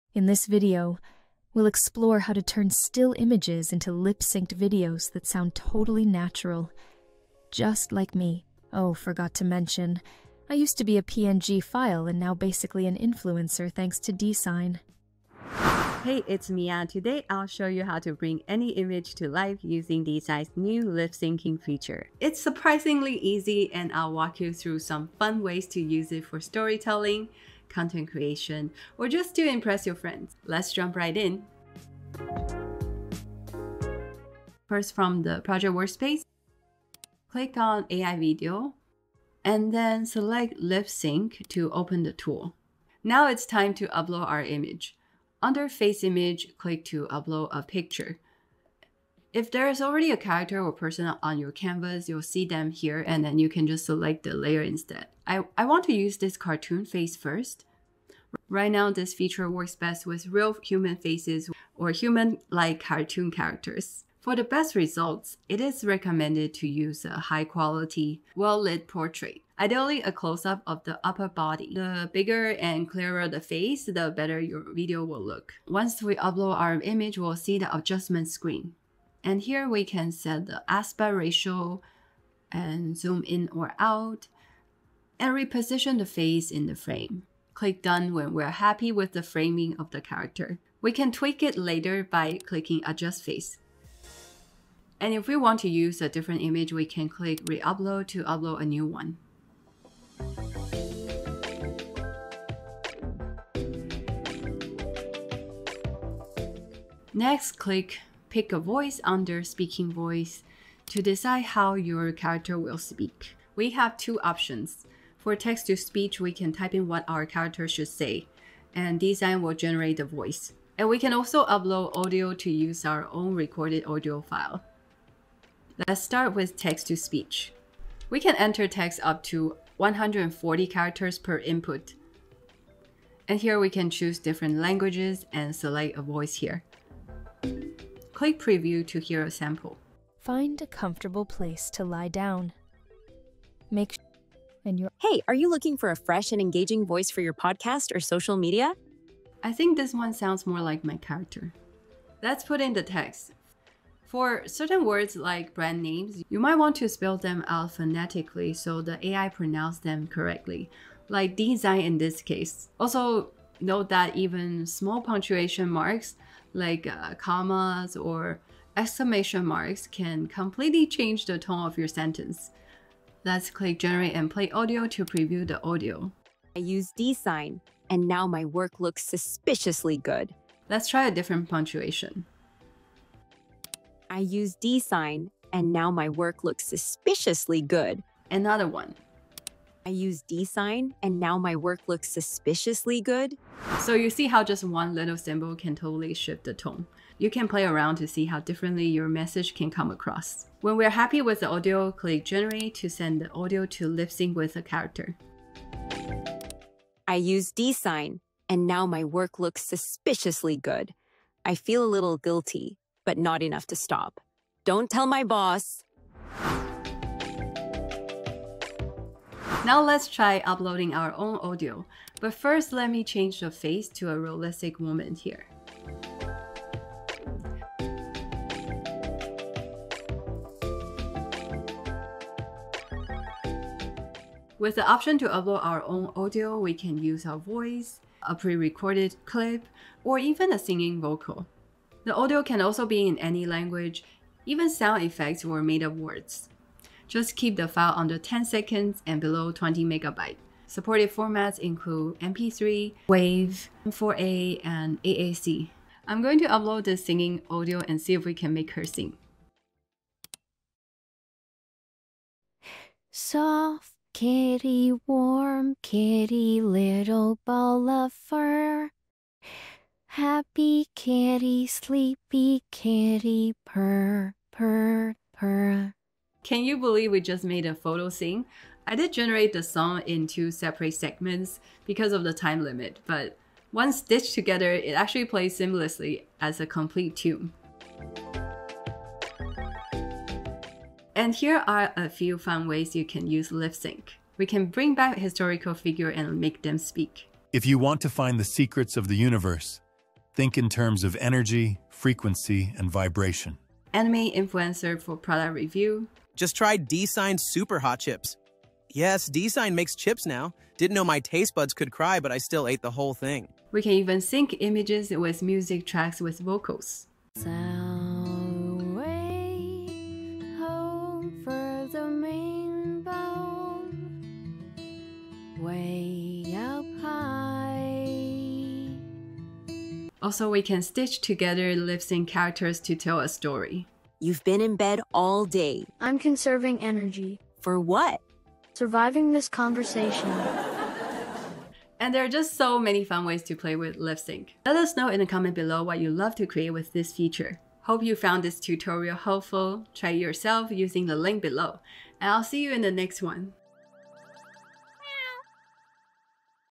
How to Make Any Image Talk Naturally (AI Lip Sync Tutorial)
In this video, I will show you how to turn any images into natural-sounding, lip-synced talking videos using Dzine’s new lip sync feature.  I will walk you through how to upload your images, adjust framing, and select or upload audio for your character. You can choose from text-to-speech or custom audio to bring your still images to life, and I will also share creative use cases for storytelling and content creation along the way.

⏳Time Travel
00:00 Intro
00:34 The Lip Sync Tool (How to Use)
01:47 Choose a Speaking Voice
03:40 Upload Audio
04:51 Creative Use Cases